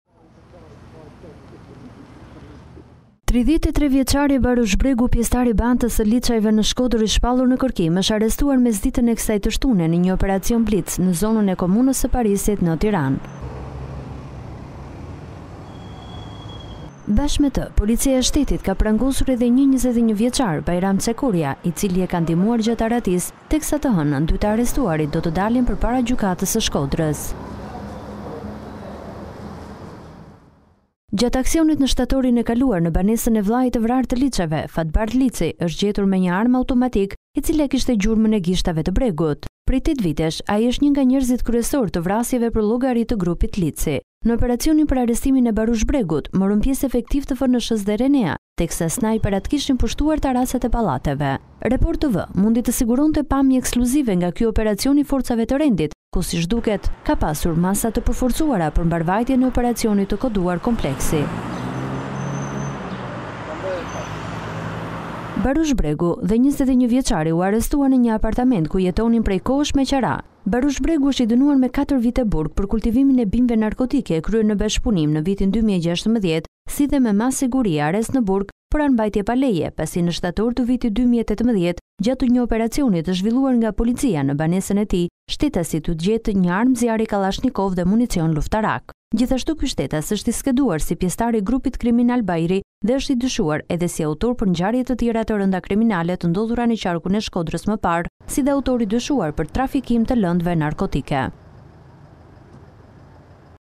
RENEA rrethon me snajper Komunën e Parisit, kapet shkodrani i grupit 'Lici', ja si u zbulua
TIRANE-Arrestohet në Komunën e Parisit një anëtar i fisit Lici në Shkodër. Ai është Barush Bregu 33 vjeç, ndërsa Report Tv ka siguruar foton e të riut që është prangosur mesditën e sotme. Bregu është i dënuar me 4 vite burg për kultivim të bimëve narkotike, e kryer në bashkëpunim në vitin 2016, si dhe me masë sigurie "Arrest në burg", për armëmbajtje pa leje.

Gjate aksionit në shtatorin e kaluar në banesën e vëllait të vrarë të Licëve, Fatbardh Lici, është gjetur një armë zjarri automatike e cila kishte gjurmët e gishtave të Bregut. Prej 8 vitesh ai është një nga njerëzit kryesorë të vrasjeve për llogarit të grupit Lici.

Dikur shok me Asmer Bilalin, tashmë i arrestuar si pjesë e grupit 'Bajri', policia thekson se Barush Bregu është i skeduar si pjesëtar i një grupi kriminal, pa specifikuar emrin, që Report Tv mësoi se bëhet fjalët për grupin e fisit Lici dhe se është  i dyshuar edhe si autor për ngjarje të tjera të rënda  kriminale të ndodhura në qarkun Shkodër më parë, si dhe autor i dyshuar për “trafikim të lëndëve narkotike".

Po gjatë operacionit  të ditës së sotme u arrestua në flagrancë dhe Bajram Çekorja 21-vjeç, banues në Shkodër për përkrahje të autorit, pasi e ndihmoi që të fshihej në Tiranë. Dy të rinjtë u arrestuan në një banesë të marrë me qira te Komuna e Parisit. Pas arrestimit mesditën e sotme në Tiranë të Bregut dhe shokut të tij, ata janë  transportuar në qytetin e Shkodrës dhe ditën e hënë do të dalin para gjykatës për t'u njohur me masën e sigurisë.

Burime pranë policisë pohojnë se operacioni 'Kompleksi' është në vijim. Ndërhyrja e RENEA-s sot zgjati një orë dhe erdhi pas 10 ditësh vëzhgim. Operacioni u ndoq nga afër nga gazetari i Report Tv, Arsen Rusta, ku u panë agjentë të RENEA-s me snajperë të vendosur në pika të larta të pallateve. Report Tv solli pamjet ekskluzive nga operacioni. Sa i përket grupit 'Bajri', ai u godit në shtator të vitit të kaluar në Shkodër dhe në Tiranë duke sjellë shumë arrestime (lexo ketu).

Në dy vitet e fundit si pasojë e përplasjes së dy fiseve në Shkodër janë shënuar disa viktima dhe të plagosur. Dy vëllezërit Astrit dhe Brahim Bilali janë dy viktimat e fundit nga seria e përplasjes. Ata u vranë në datë 30 mars, ndërsa në 26 prill u arrestuan dy persona të cilët akuzohen se kanë ndihmuar ekzekutori kryesor, Ibrahim Lici, i cili është shpallur në kërkim.